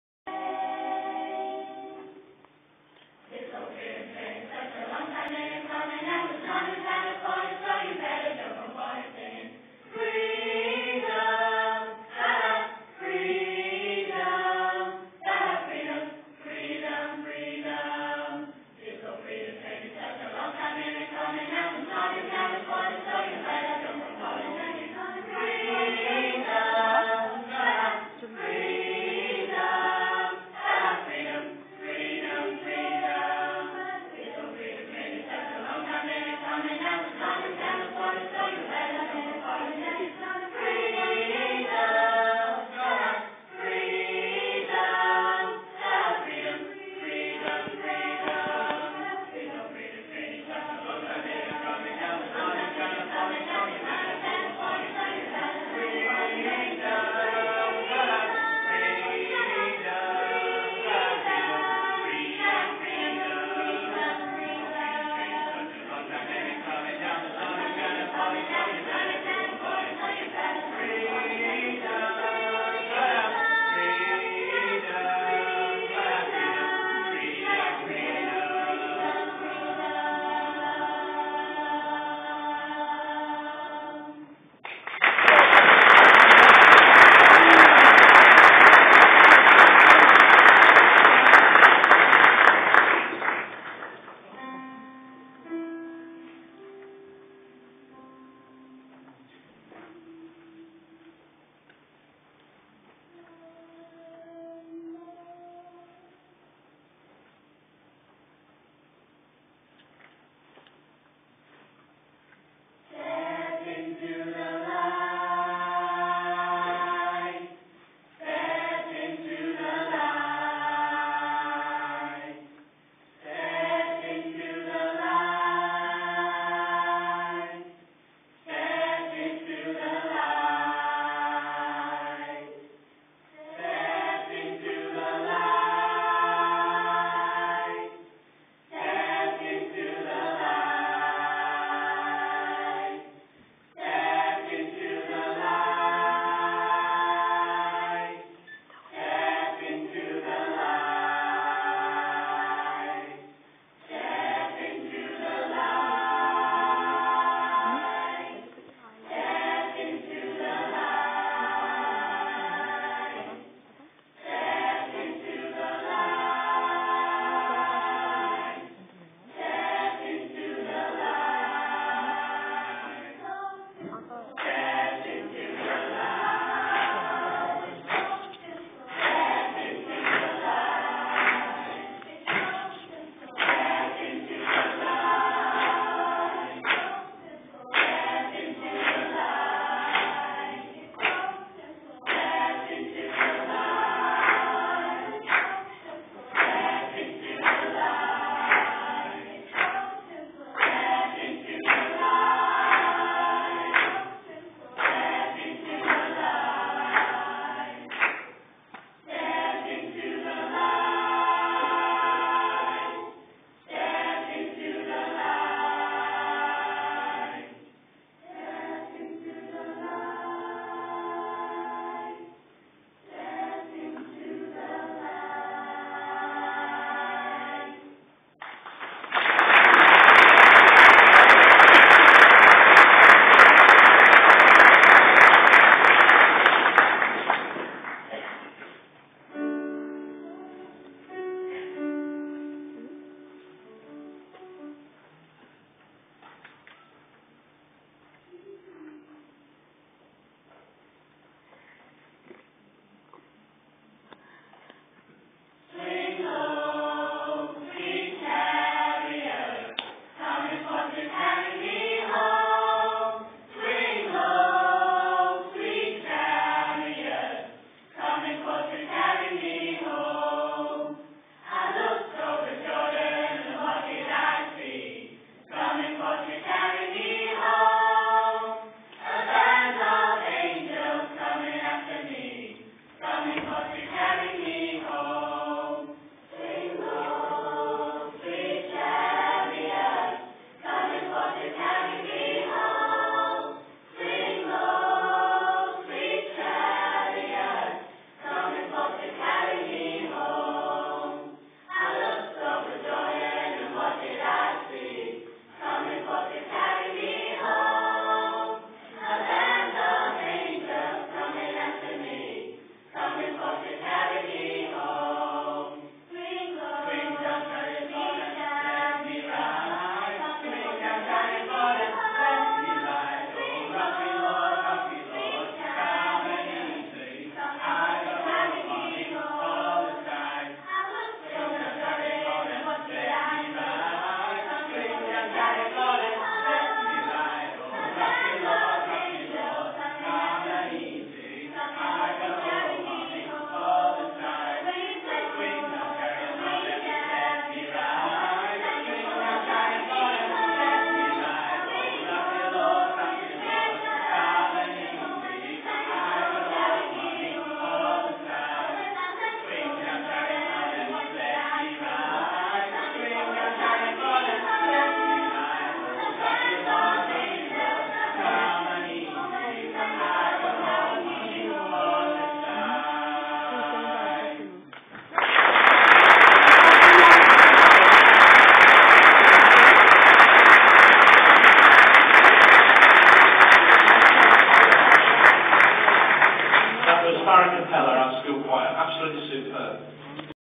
farringdon community sports college choir
farringdon community sports college choir (farracapella) singing at the governors awards 2009